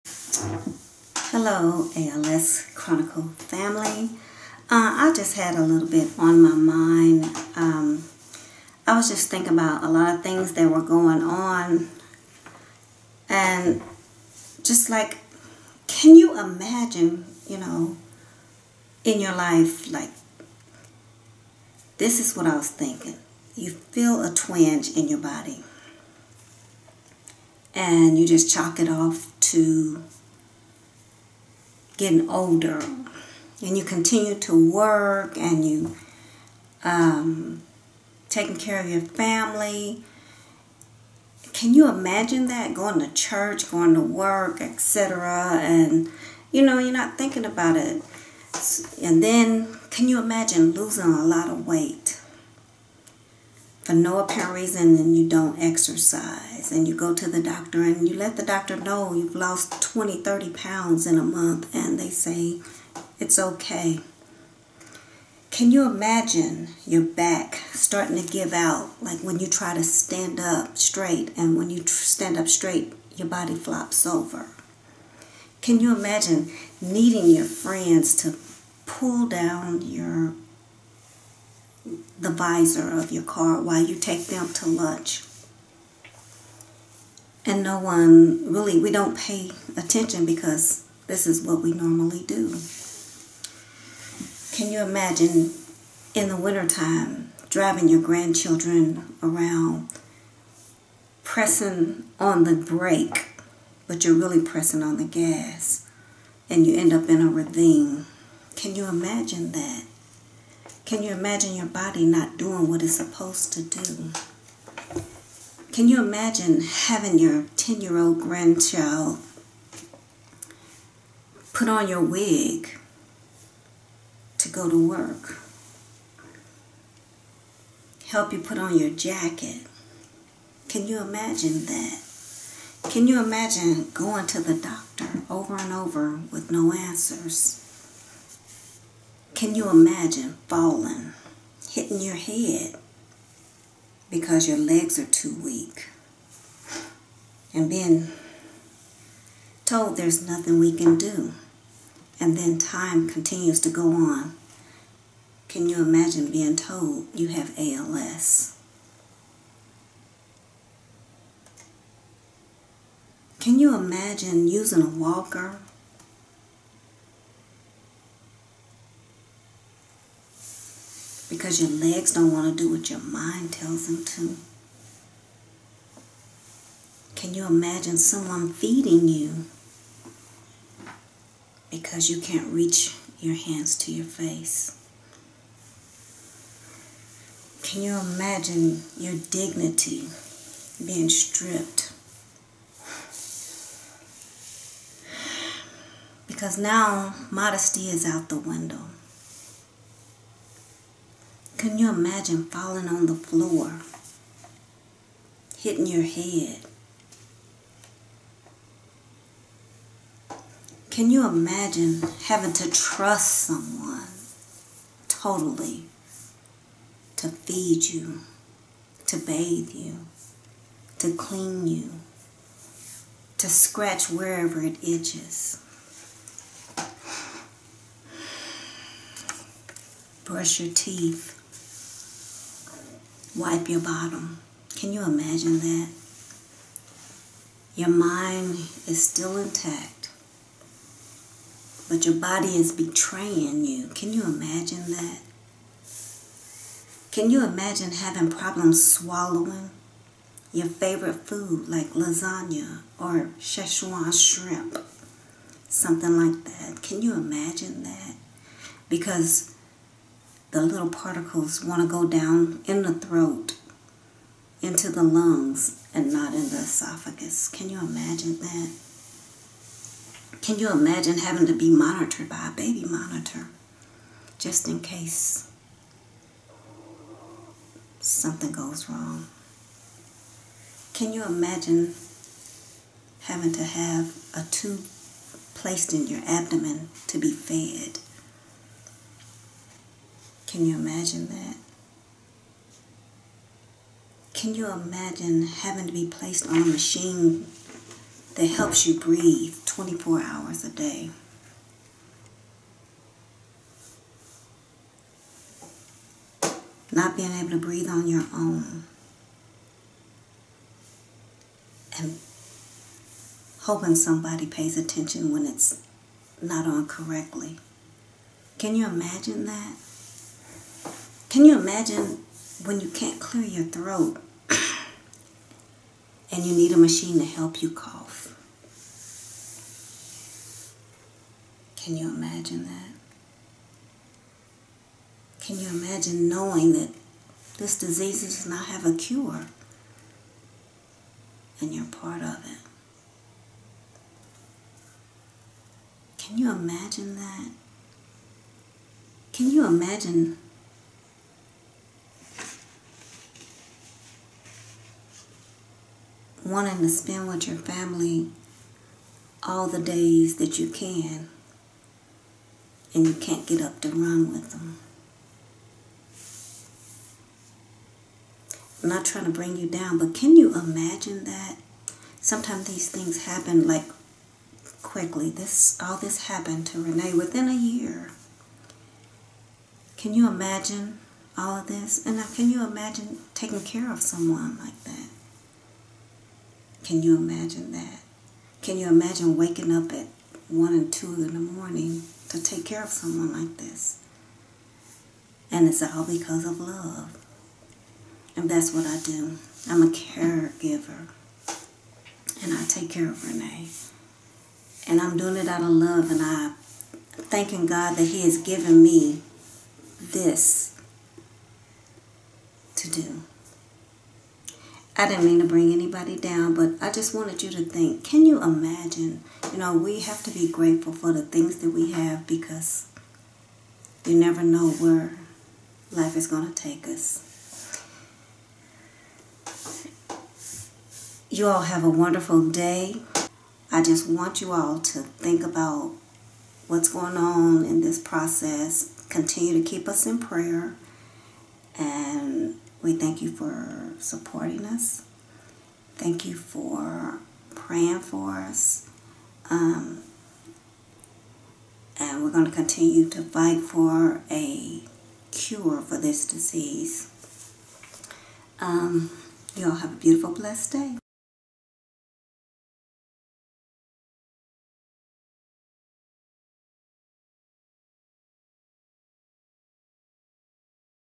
ALS Chronicles - Can You Imagine...?
Having unexpected changes in your physical body can be overwhelming and frightening. Can you imagine having changes and not knowing what's happening and then find out you have ALS which has no cure. You know your body will continue to fail with your mind remaining lucid. Can you imagine?